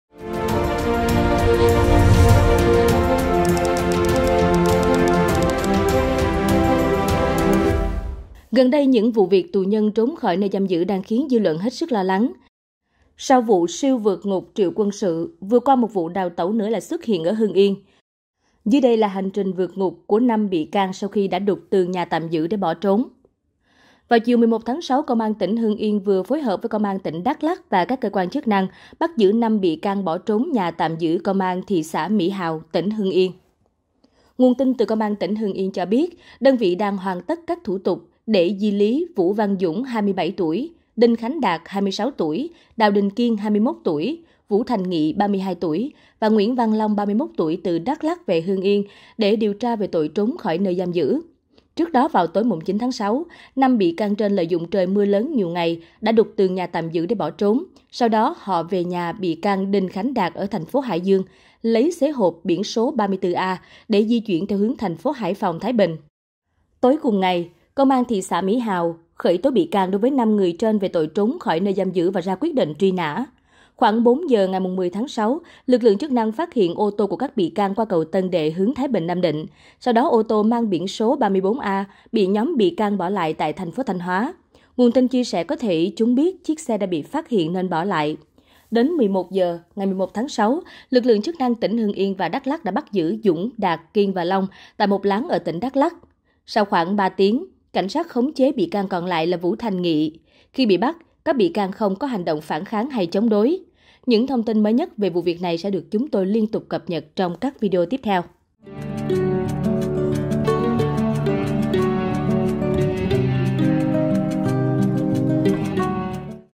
Hành Trình Lẩn Trốn Của 5 Bị Can Trốn Khỏi Nhà Tạm Giam Hưng Yên | SKĐS
SKĐS | Gần đây, những vụ tù nhân trốn khỏi nơi giam giữ đang khiến dư luận hết sức lo lắng. Sau “siêu vượt ngục” Triệu Quân Sự, vừa qua, một vụ đào tẩu nữa lại xuất hiện ở Hưng Yên. Dưới đây là hành trình “vượt ngục” của 5 bị can sau khi đã đục tường nhà tạm giữ để bỏ trốn. Hãy cùng chúng tôi theo dõi xem 5 đối tượng này sau khi trốn khỏi trại giam đã đi đâu, làm gì.

Mời quý vị và các bạn xem thêm: Vụ Bao Tải Trôi Trên Sông Mã: Mẹ Nạn Nhân Tiết Lộ Tin Nhắn Cầu Cứu Của Con Trai